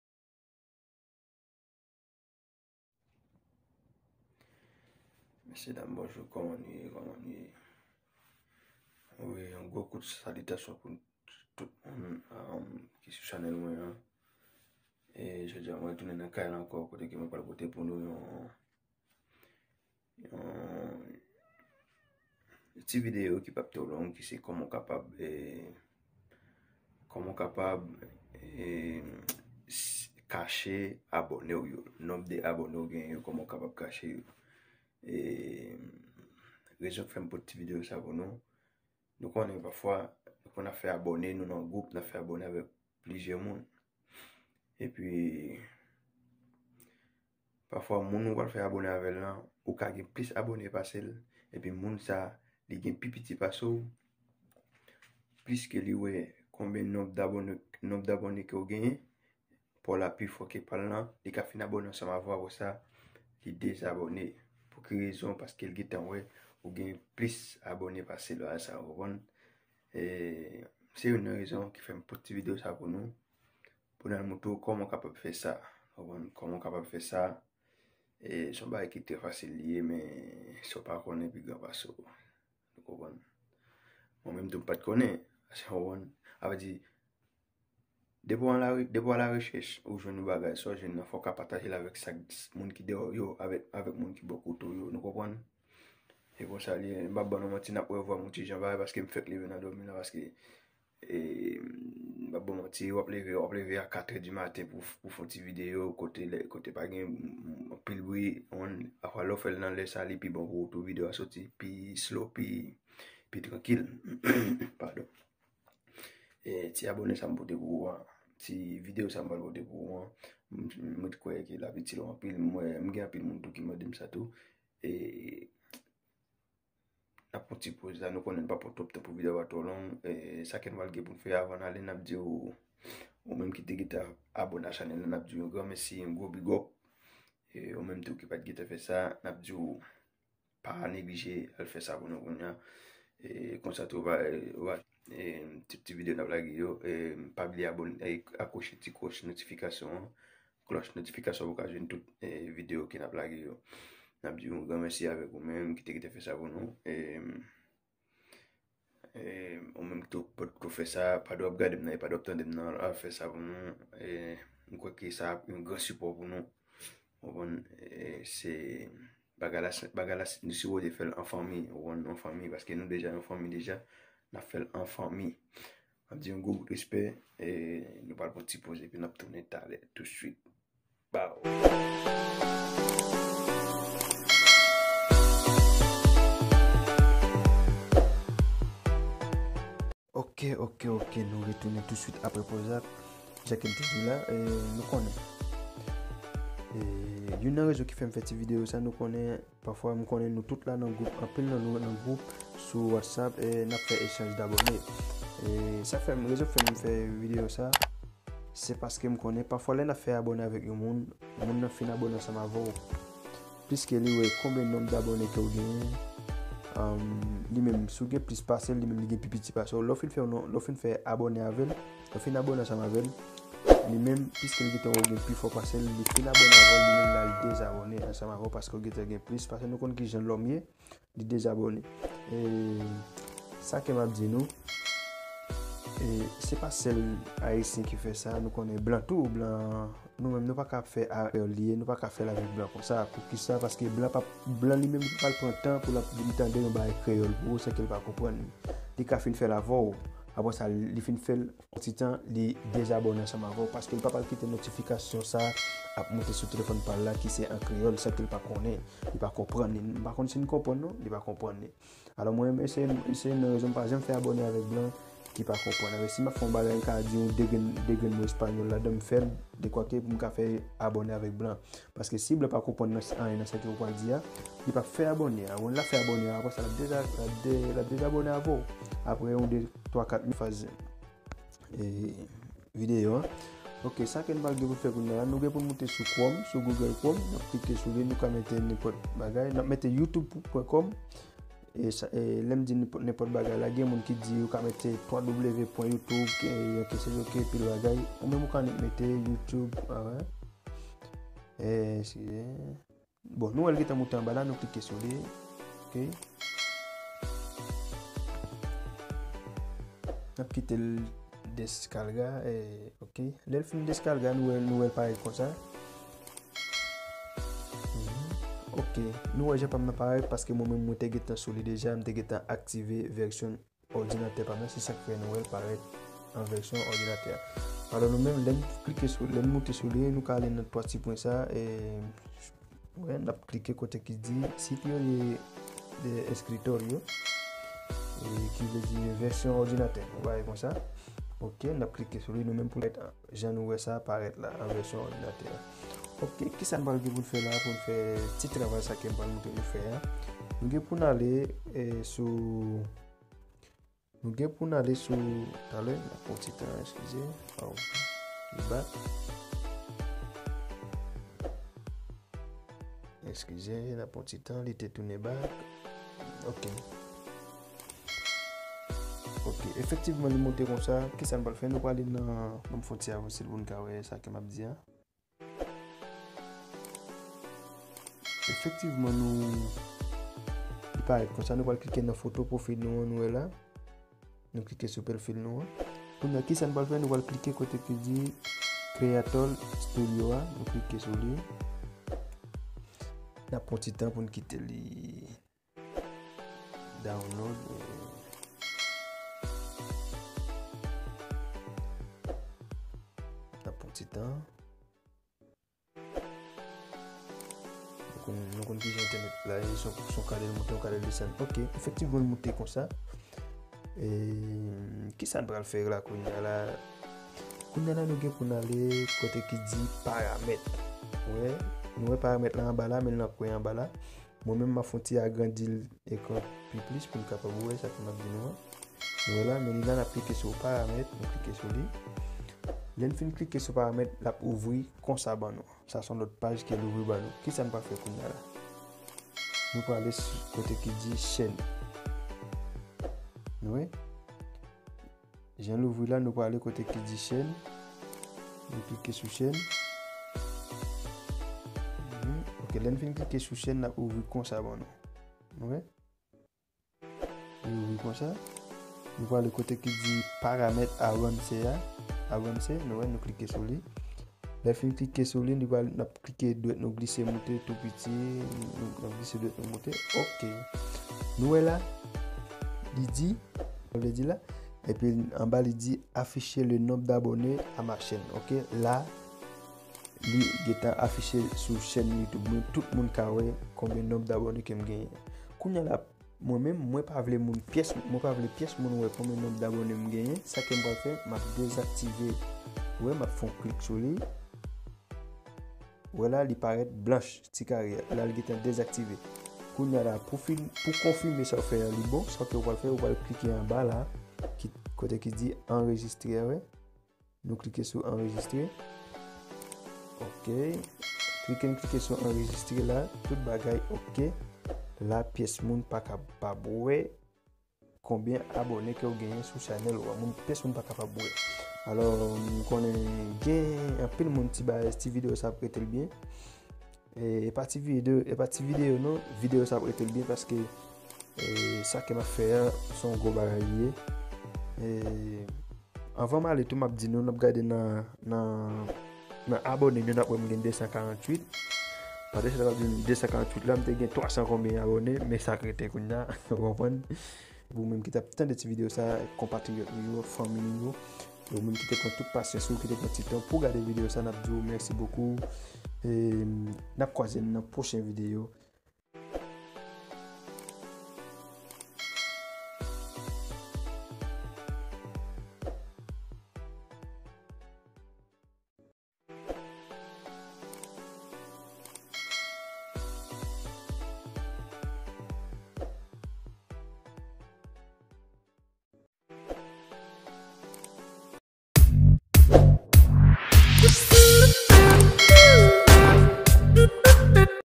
Fason pou mete abone'w yo prive pou moun paka we yo
Men kijan pou kache  aboné ou genyen sou Youtube pou moun pa we konbyen aboné ou genyen